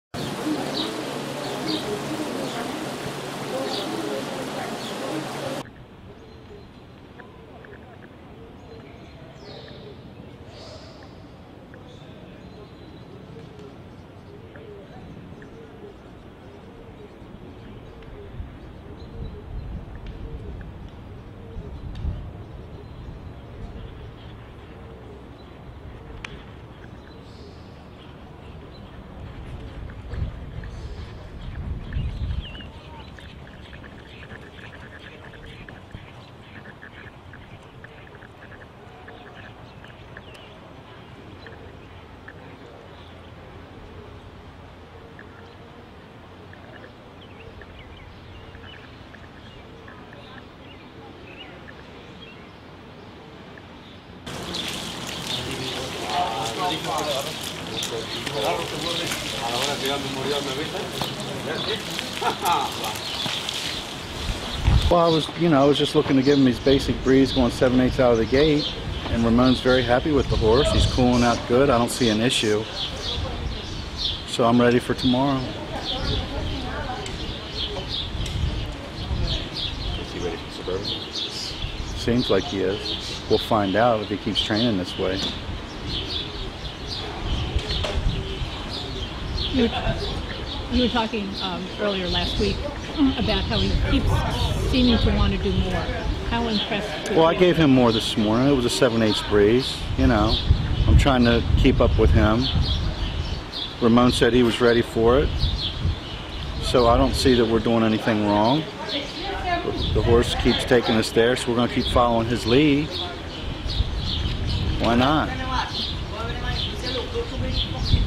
I Want Revenge breezes June 17 at Big A
I Want Revenge moves a step closer to returning to the races with a 7 furlong breeze